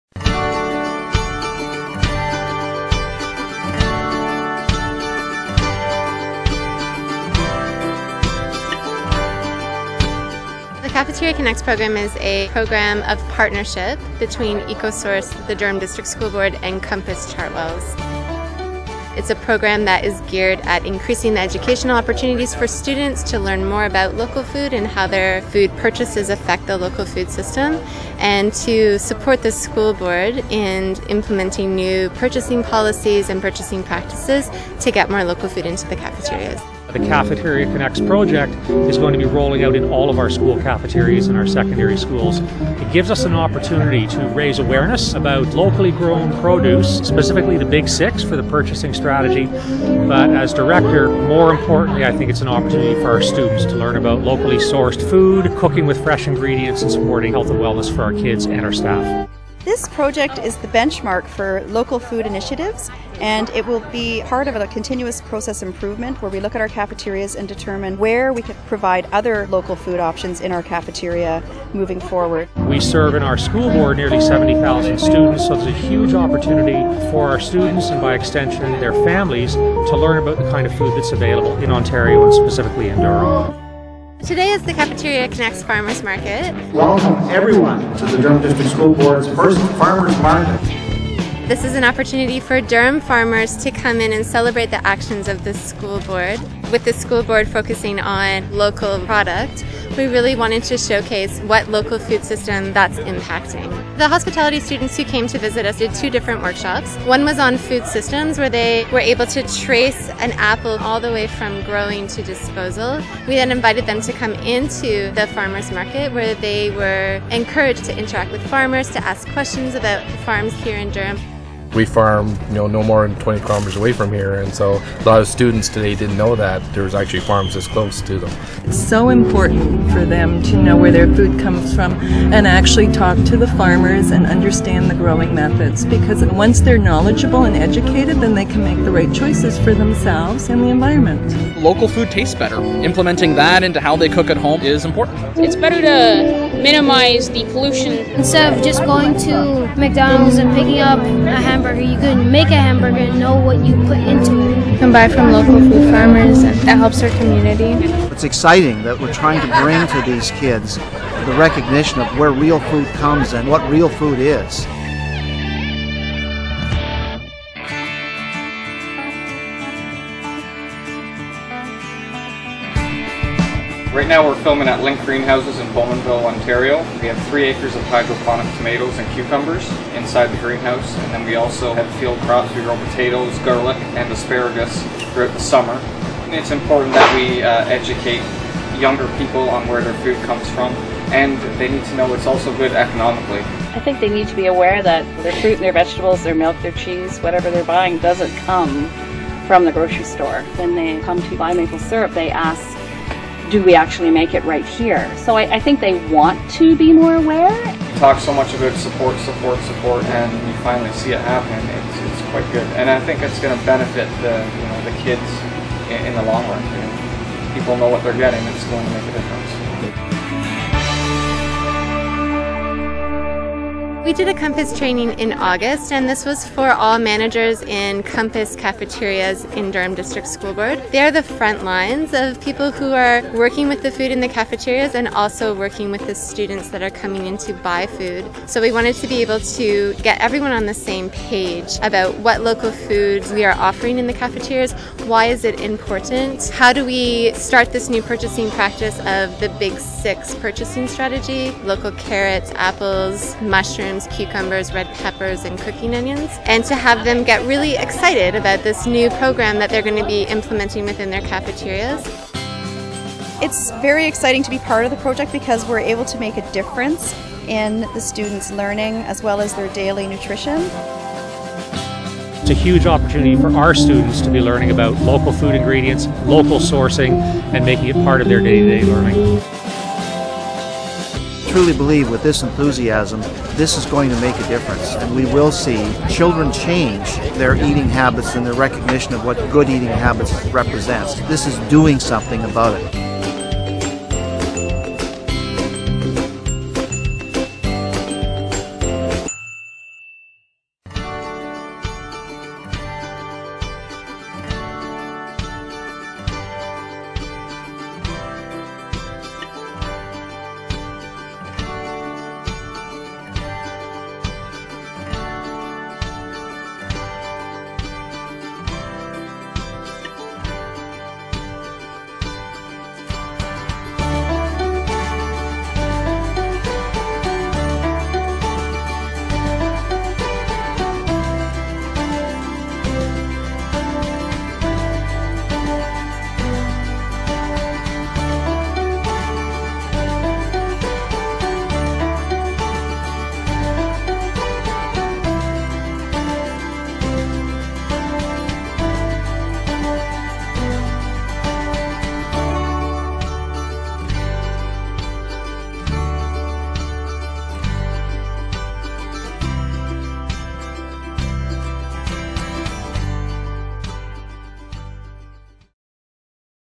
ecosource - Classrooms Connect with Local Food
The DDSB got a healthy boost with the release of the new Classroom Connects Local Food Curriculum Guide